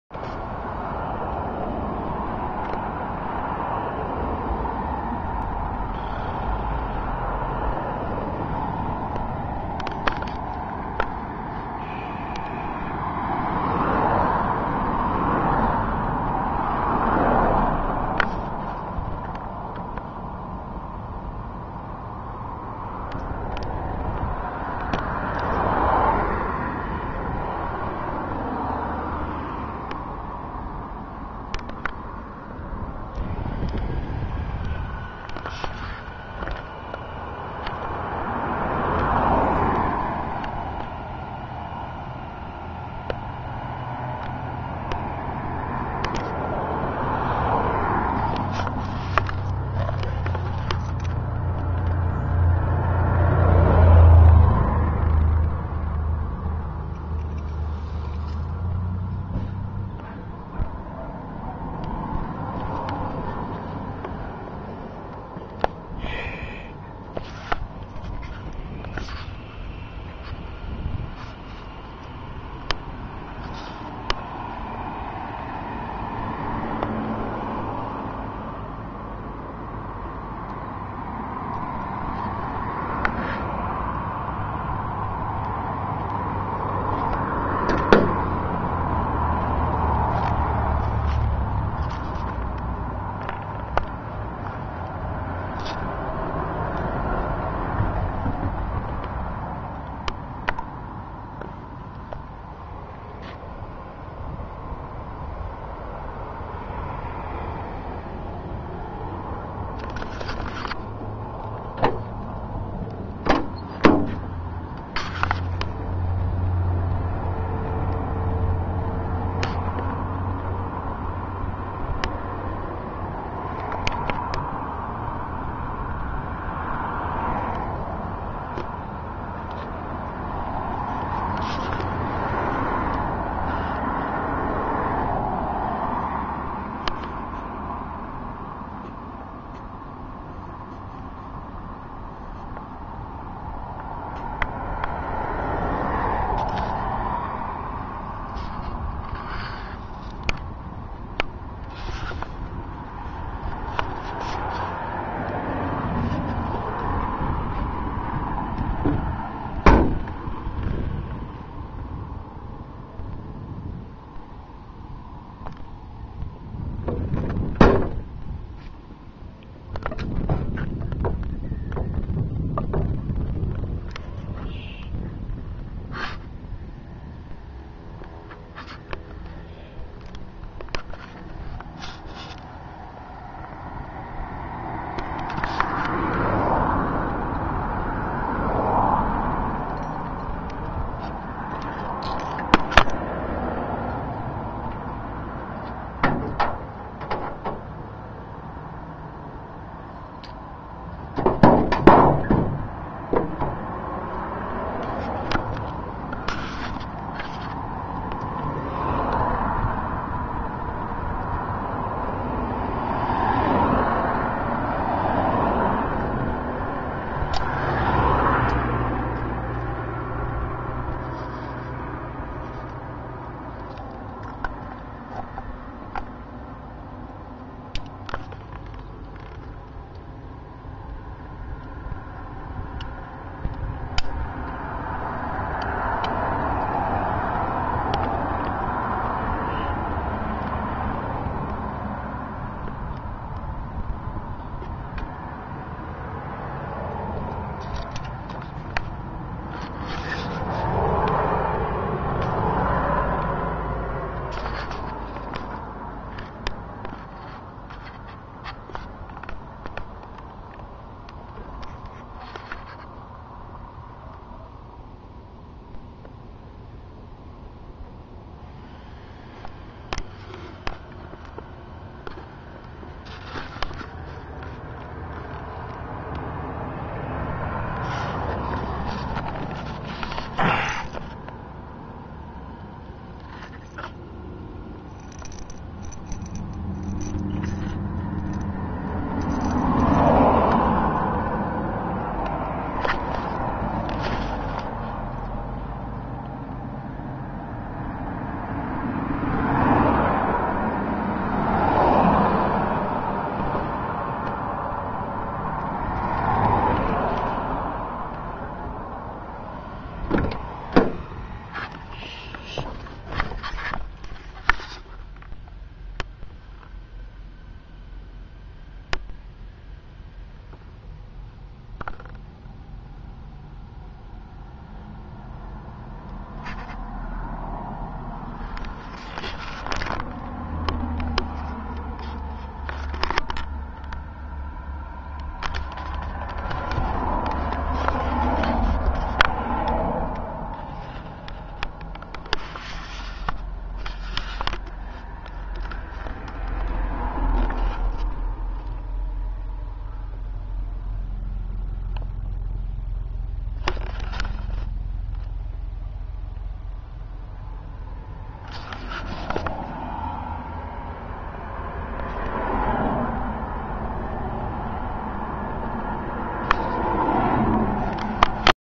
beater datsun 510
had lots of fun with car until the headgasket blew